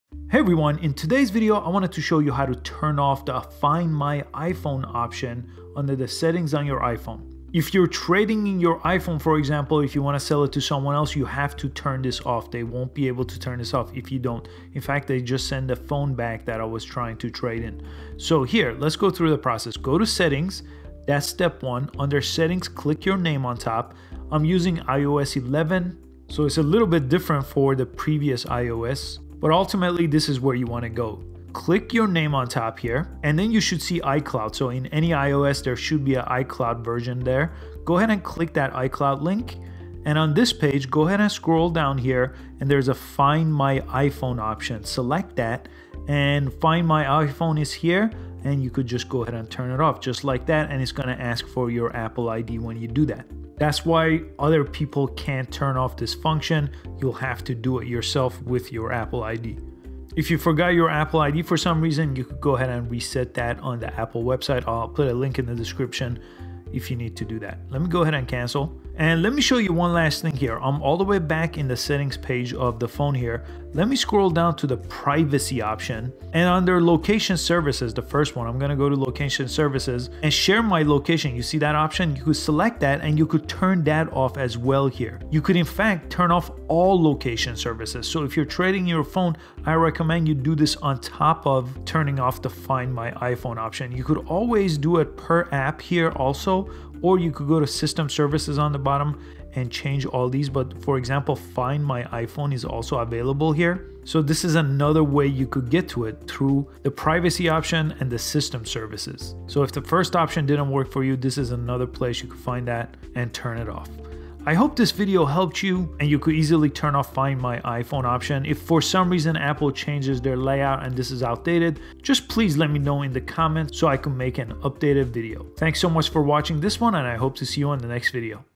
How to Turn Off Find My iPhone
You can easily turn off find my iPhone in your iPhone settings.

You can find this option in two places.  The first place is under the iCloud option and the second location is under location services that are under privacy.

Make sure the option is turned off in both locations.  You can turn off location services entirely as well after you turn off find my iPhone.